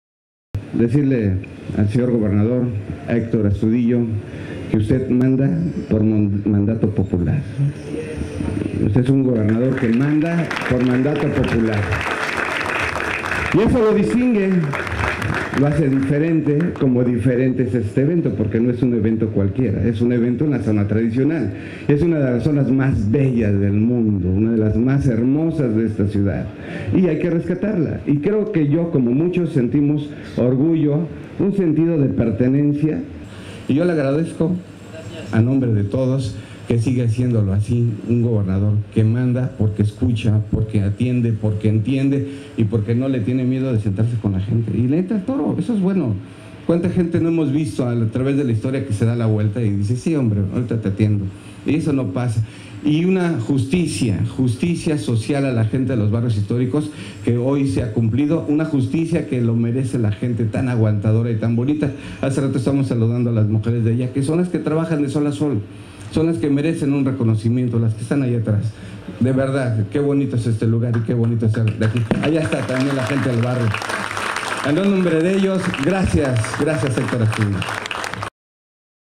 GradoCeroPress MANUEL ZAMUDIO GERENTE TELEVISA CALLES ACAPULCO TRADICIONAL
Conoce las nuevas calles rehabilitadas por Gobierno del Estado en Zona Tradicional

Acapulco, Gro.- Con una inversión de 38 mdp, el gobernador Héctor Astudillo Flores acompañado de Rafael Navarrete Quezada, titular de Desarrollo Urbano, Obras Públicas y Ordenamiento Territorial, inauguró la rehabilitación de las calles Palmas, Gran Vía Tropical y Adolfo López Mateos, vialidades aledañas a Playa La Angista, lugar de gran importancia para el turismo, comerciantes y familias residentes, ubicada en la zona tradicional de Acapulco.

Esta obra que se suma a las realizadas, por empresas 100% guerrerenses, durante esta administración; sumándose a la  rehabilitación con concreto de estampado colonial de las calles Hornitos, Patal, José Azueta, Quebrada, La Pinzona, Urdaneta, Lerdo de Tejada, Castillo Bretón, José Valdéz Arévalo, Vasco Núñez de Balboa y La Pocita en la Zona Tradicional del puerto.